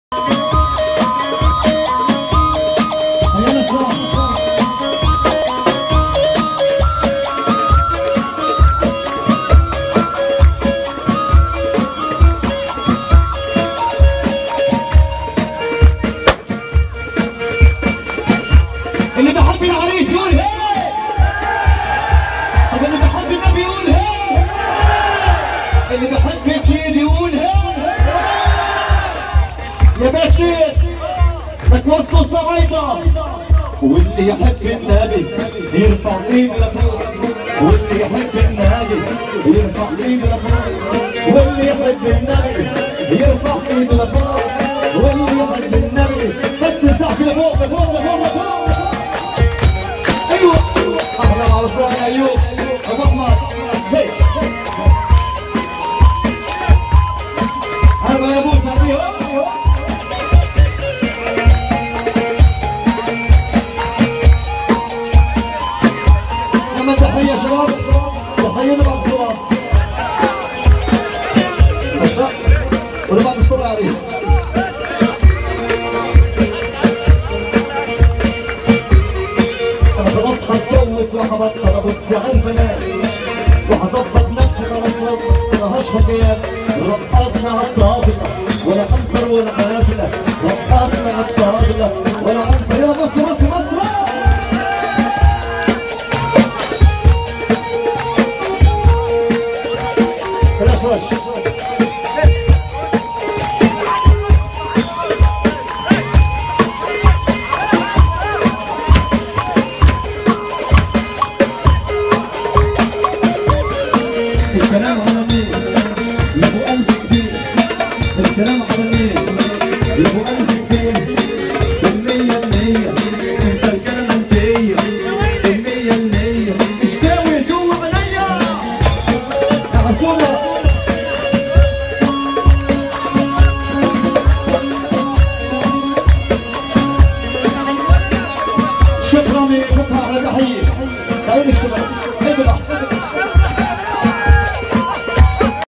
جديد ومهرجان بيت لِيَد والفنان الفلسطيني حموده حنون -٢٠١٧
مصري نار من حفلة بيت لِيَد والفنان حموده حنون